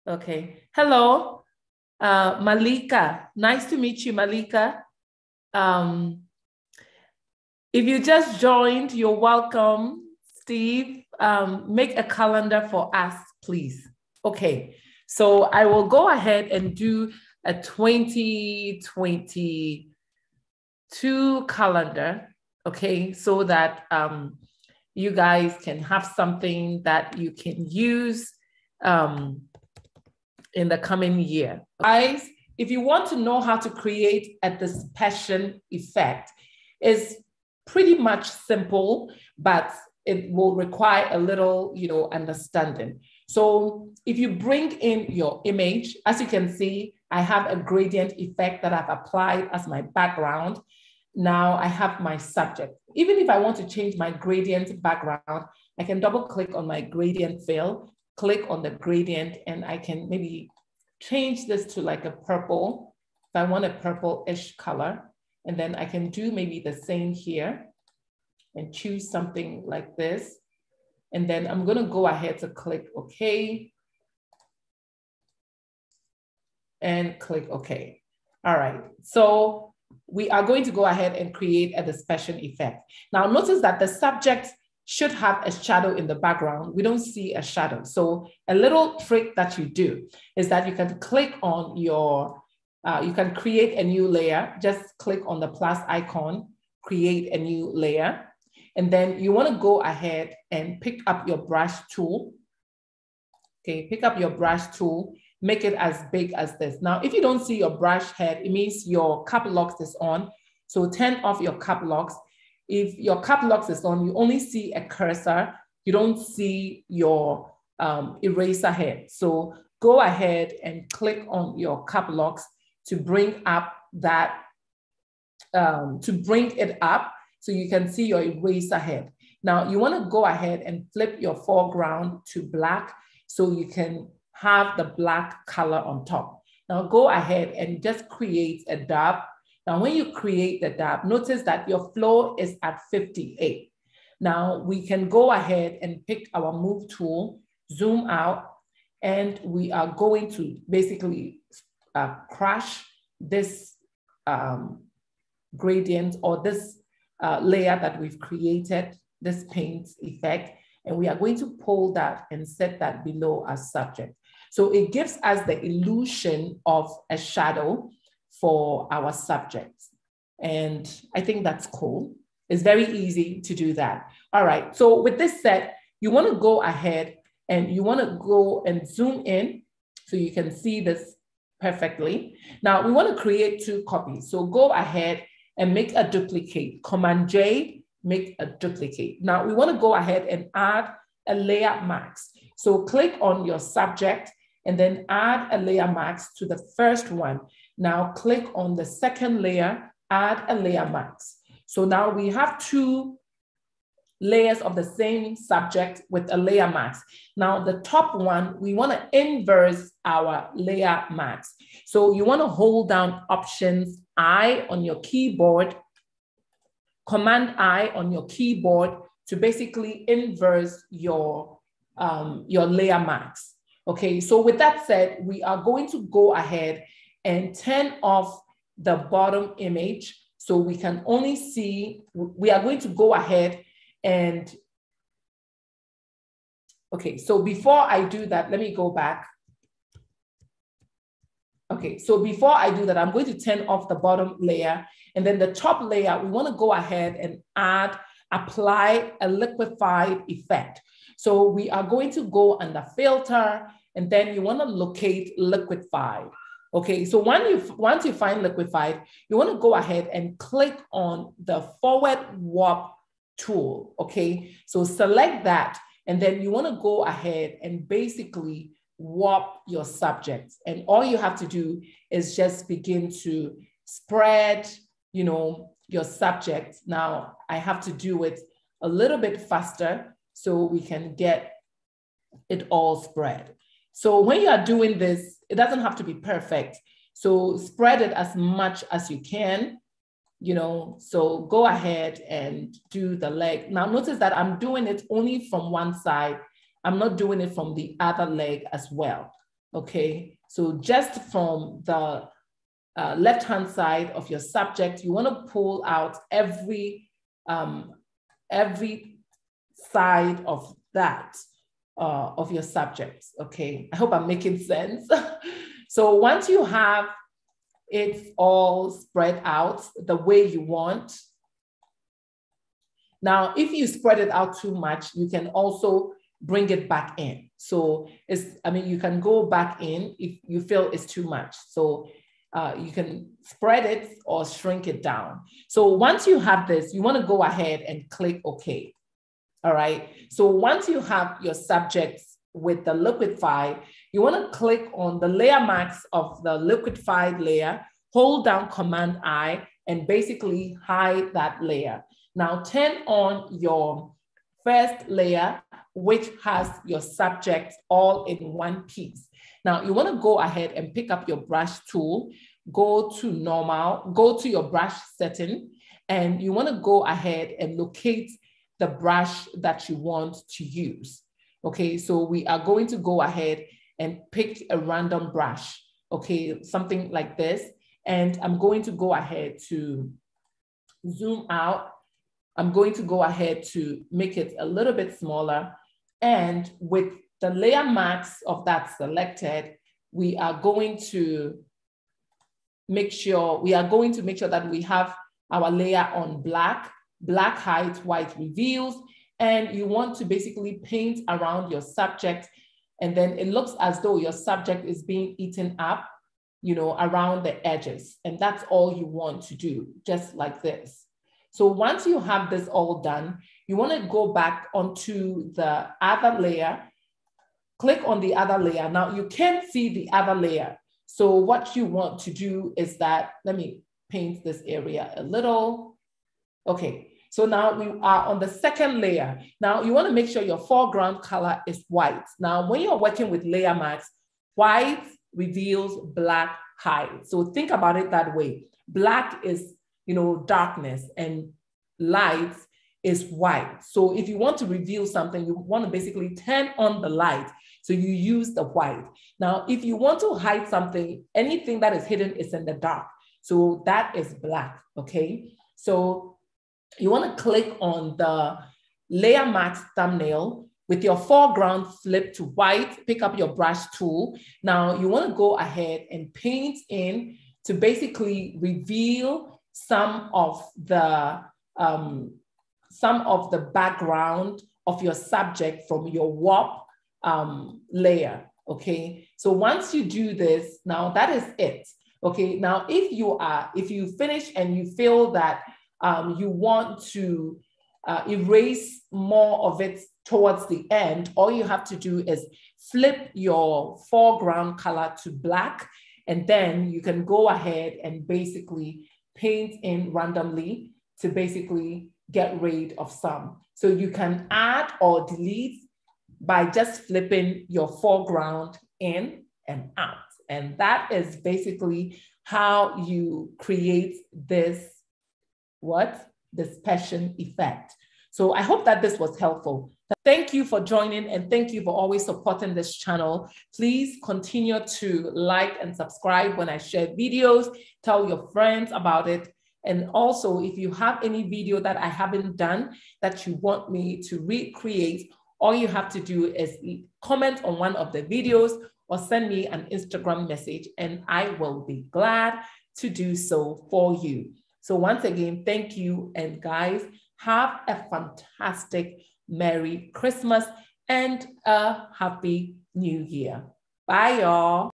Dispersion effect in Photoshop | Adobe Photoshop for Beginners | Quistt Live!
In this photoshop tutorial, I will show you how to create a dispersion effect in Photoshop

//D O W N L O A D    T U T O R I A L    I M A G E S